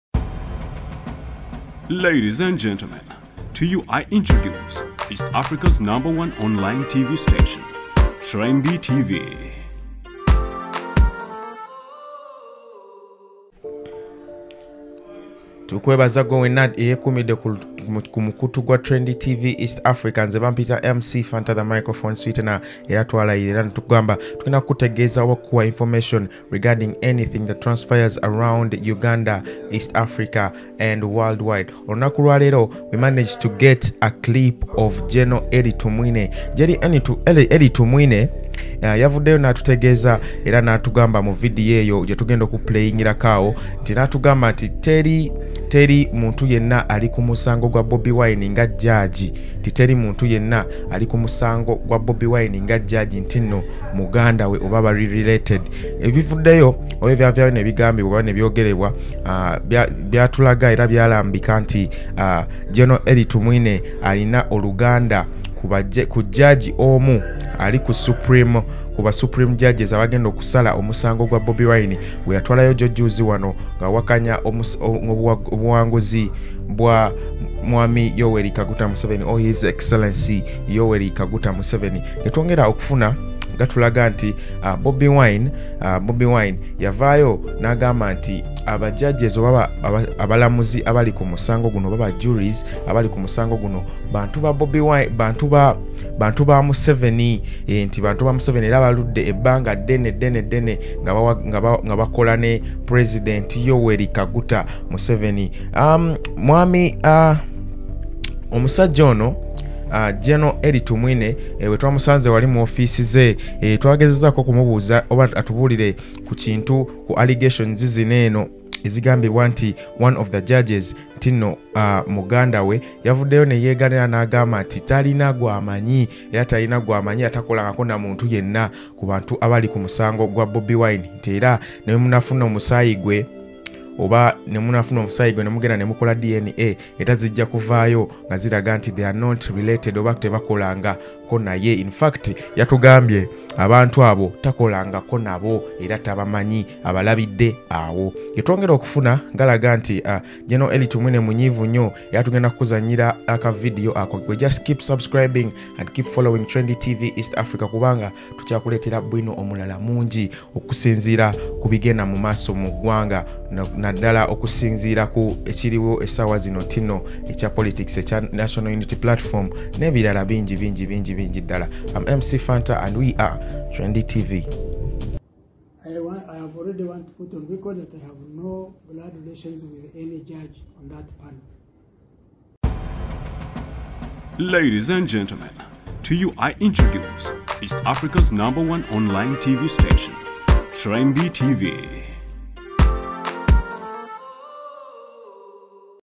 Gen. Elly Tumwine Warns Bobi wine ~ I Have No Blood Relations With Any Justice On Supreme court
VIDEO: "I have no blood relations with any Justice on the Supreme Court panel as alleged by blackmailers. I forgive them" Gen Tumwine.
Bobi Wine asked three justices to disqualify themselves from hearing his election petition against Museveni, citing conflict of interest.
“We have questions about the Chief Justice Owiny-Dollo, having been a former Museveni minister, having been a former Museveni defence lawyer and also we have seen pictures indicating that the Chief justice has been meeting with Gen Museveni on a regular basis. We believe that within the law, that is wrong. We also know that (Justice) Ezekiel Muhanguzi is a relative of Gen Eli Tumwine, a person that the national and international community has castigated for abusing human rights,” Bobi Wine sad